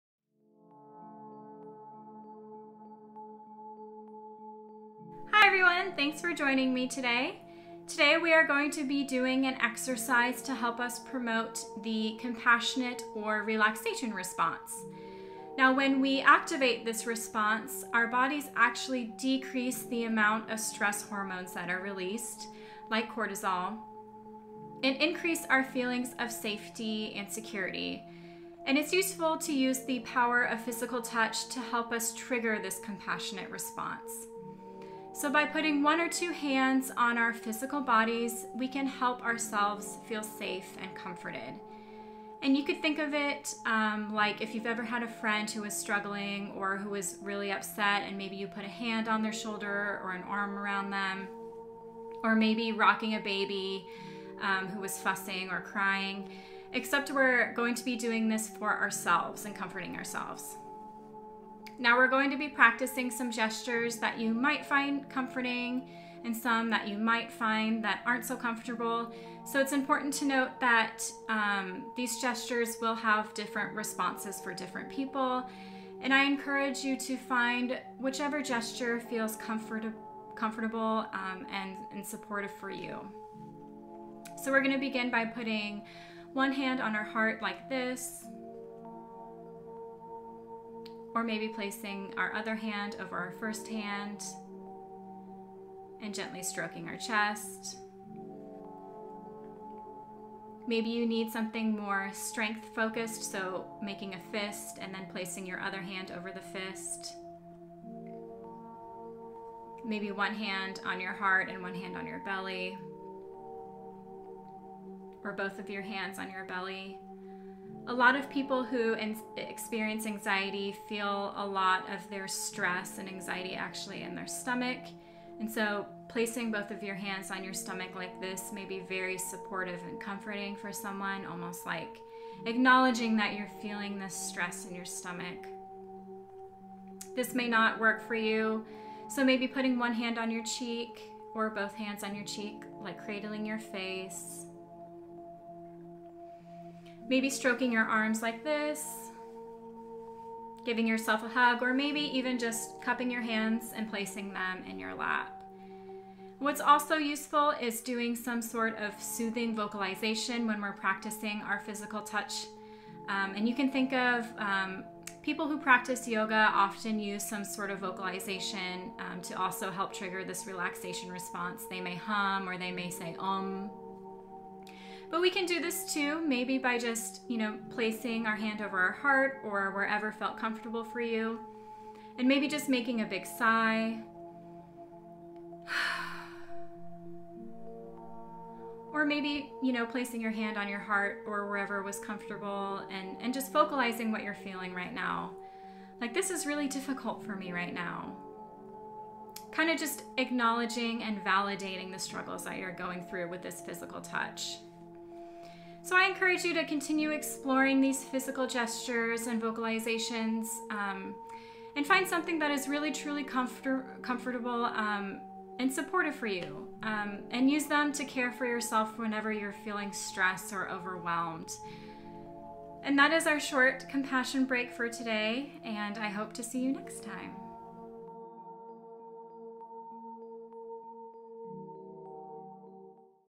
Self-Compassion Check-in | Physical Gestures
We can comfort ourselves during a stressful time by using a supportive, gentle touch and soothing vocalization.

Source: Neff, K., & Germer, C. (2018). Soothing Touch. In The Mindful Self-Compassion Workbook: A Proven Way to Accept Yourself, Build Inner Strength, and Thrive (p. 33). New York, NY: The Guilford Press.